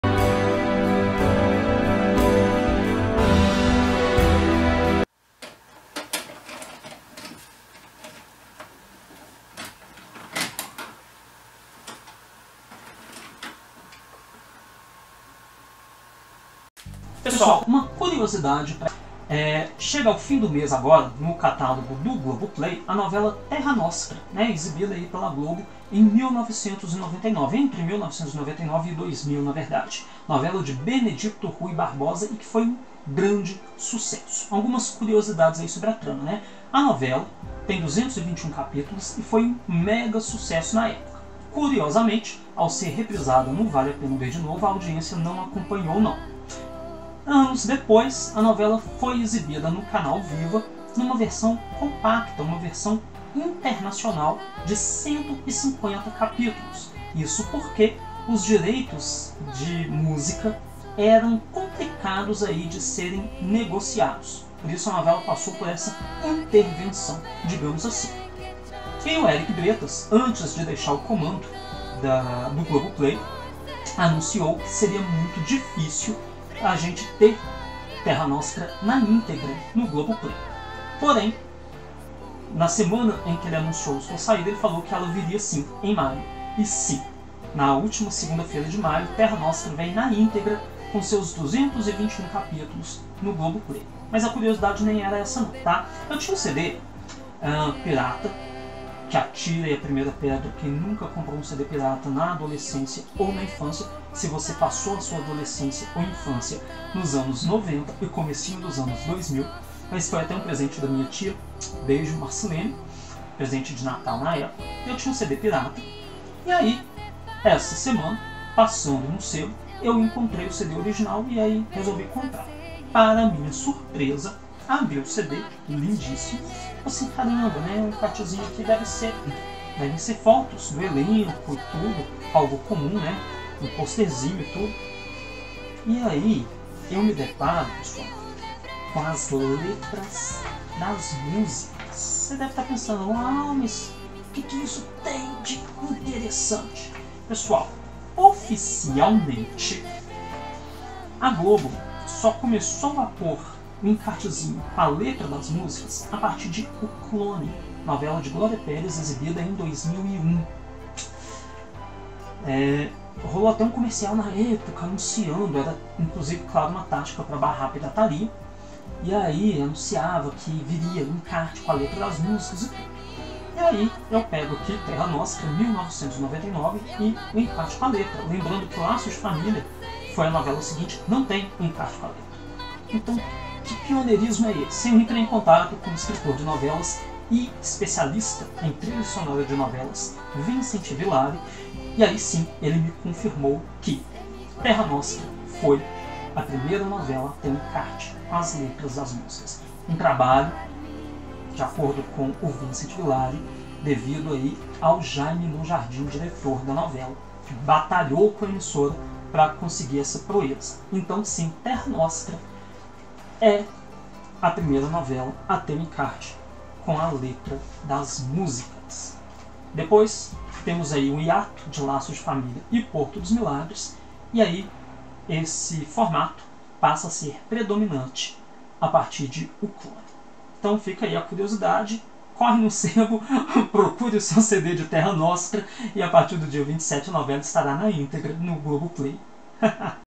Terra Nostra foi a primeira telenovela da Globo a ter encarte com letra de música no CD
Confira essa e outras curiosidades da novela que chega no catálogo do Globoplay na íntegra em 27/05.